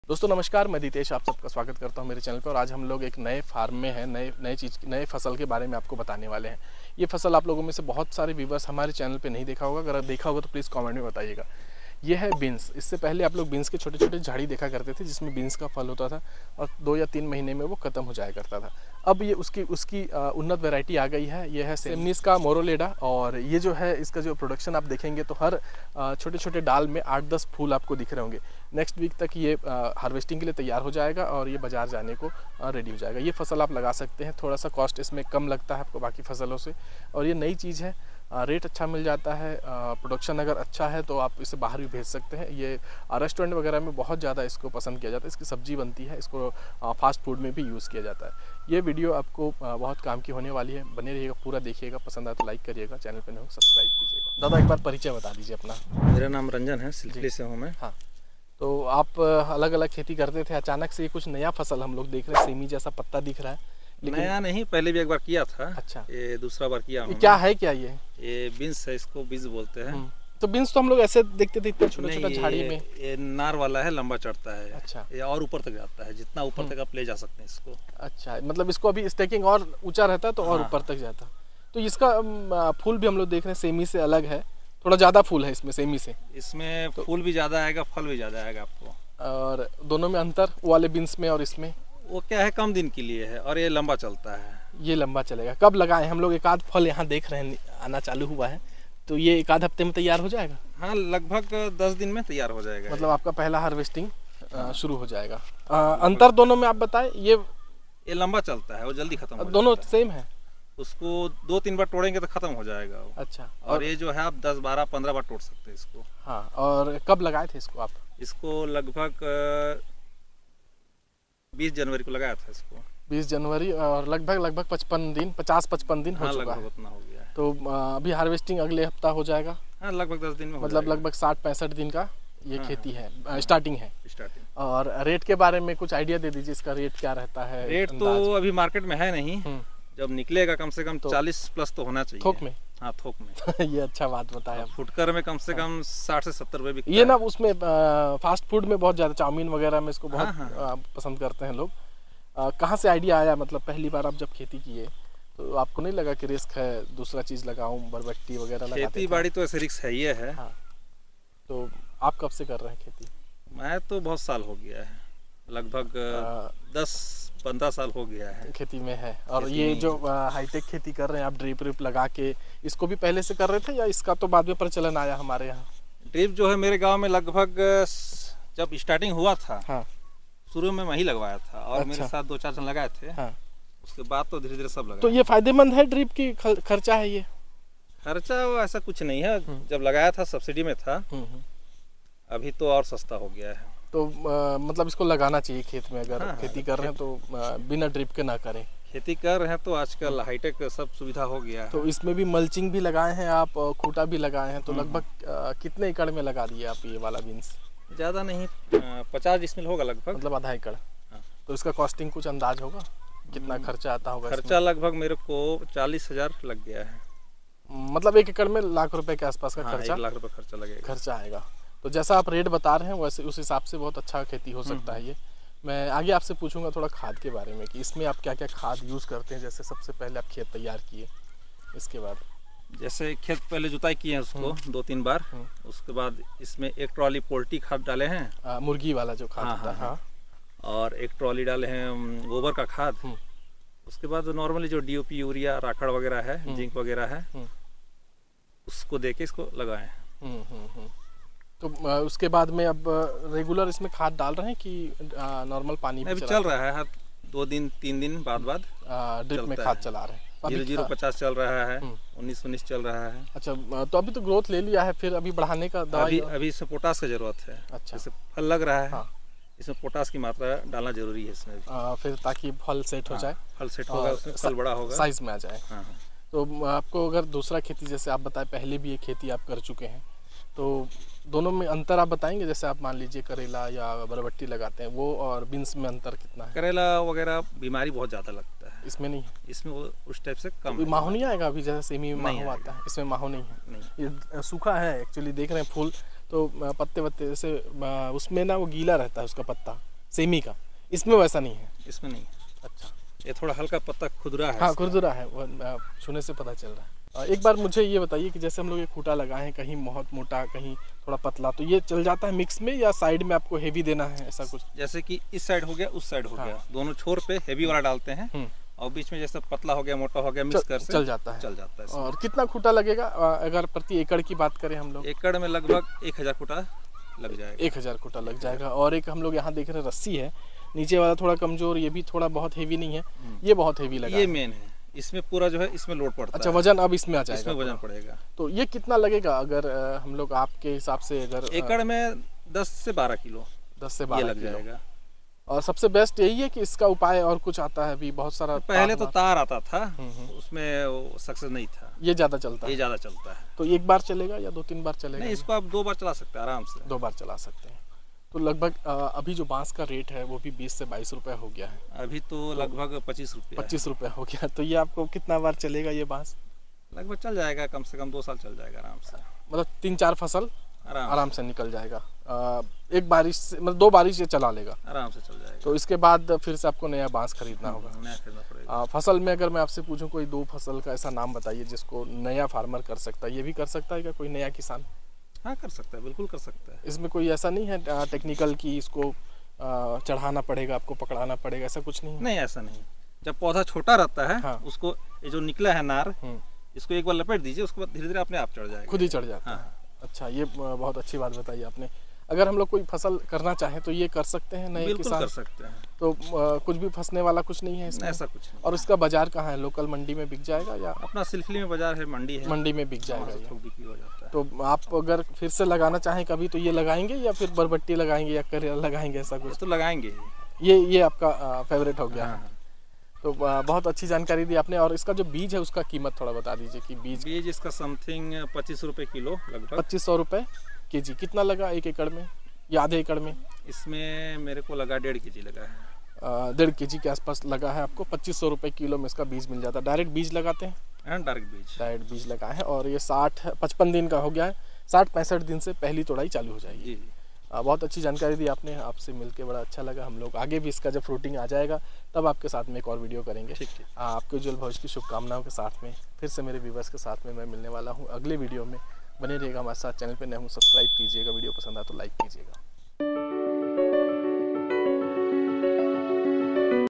इस फसल से किसान होंगे मालामाल👌 | गर्मी के लिए सबसे बेहतर फसल
बींस की खेती आधुनिक तरीक़े से